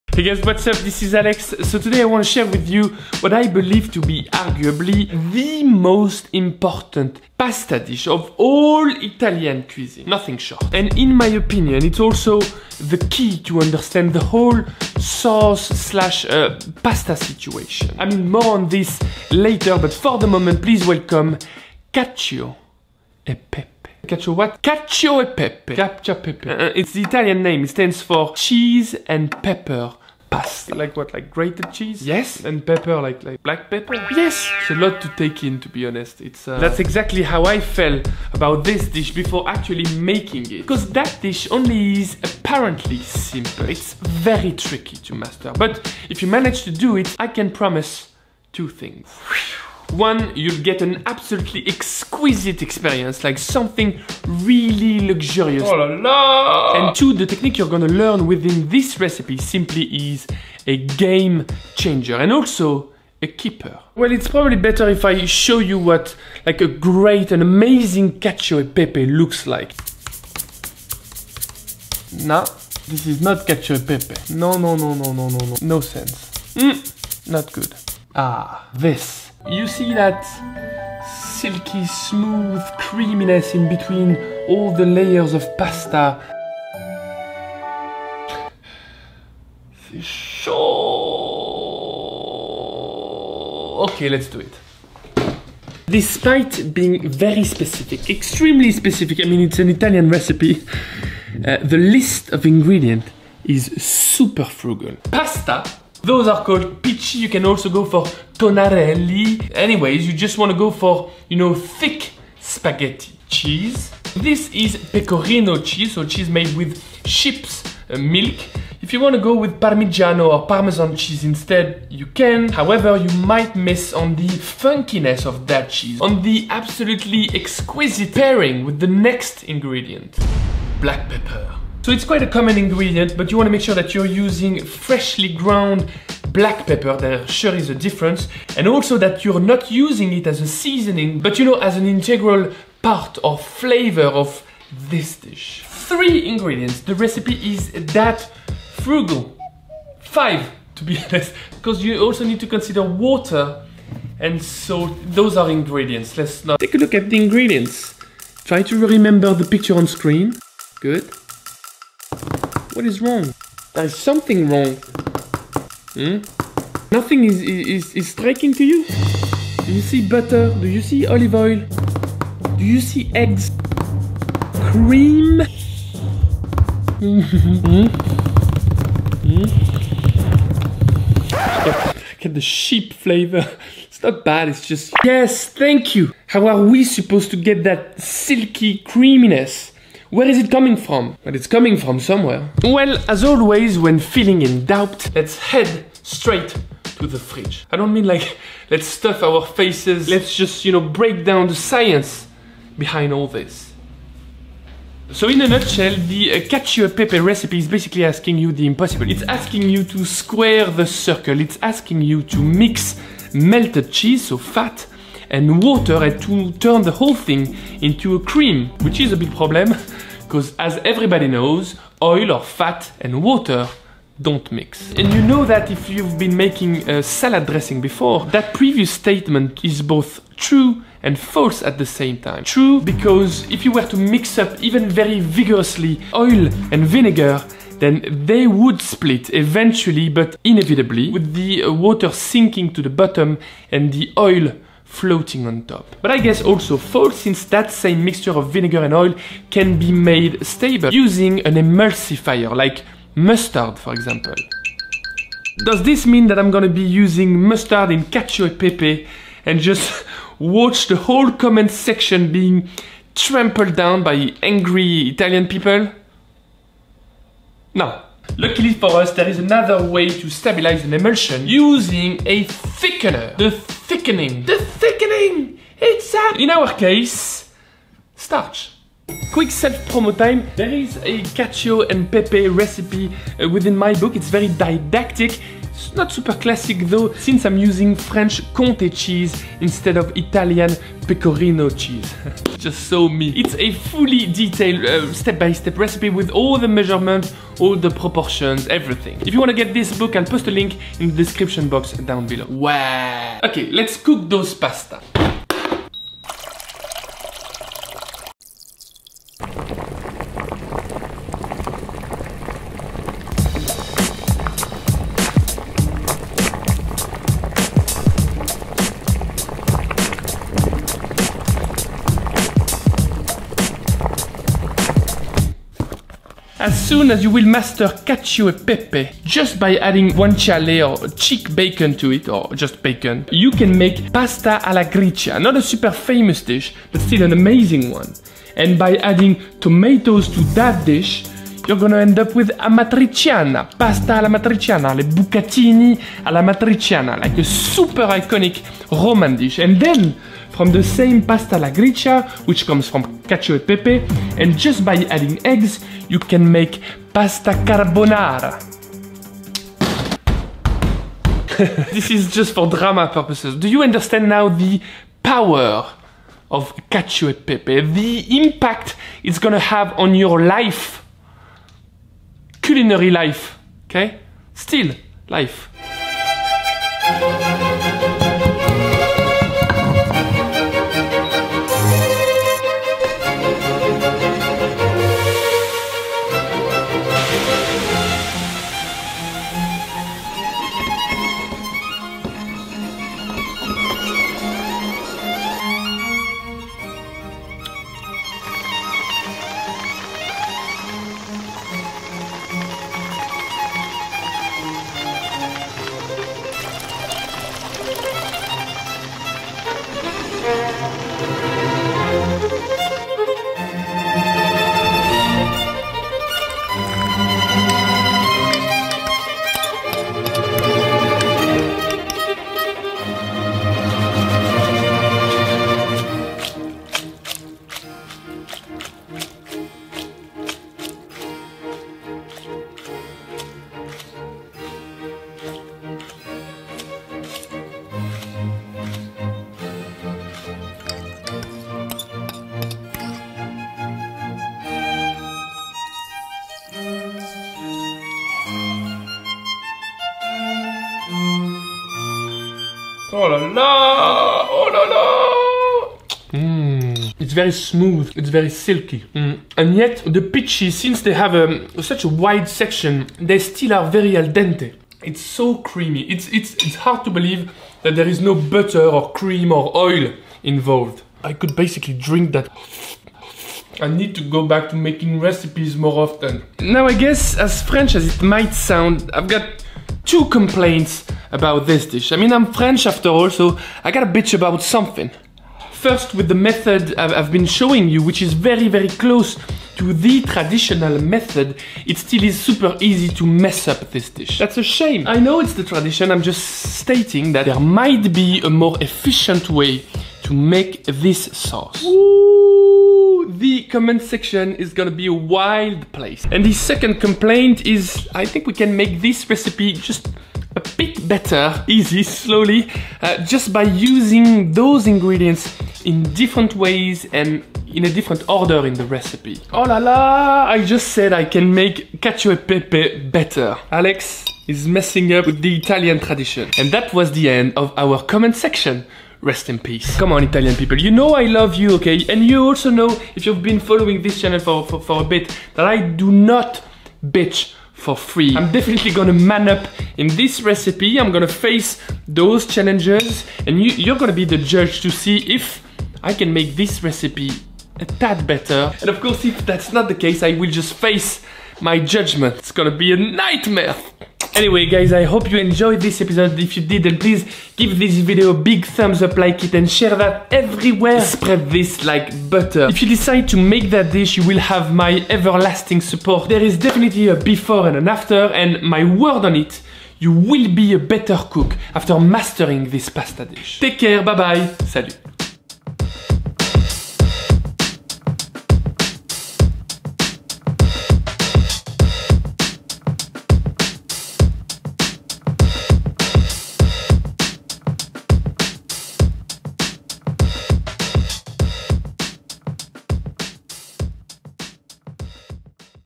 This Italian Pasta Dish Had A Tremendous Impact On Me... (Cacio e Peppe)
No butter - No cream. True "Cacio E Pepe" only requires Pasta, Pecorino cheese and Black Pepper. Still the creamiest Pasta dish ever. My cookbook

Classic pasta for this Roman Cacio e Pepe dish include : Tonnarelli or Pici. Parmesan cheese can be use but at your own (diplomatic) risks. Black pepper is not only a seasoning, it's a main actor.

Salut,

Alex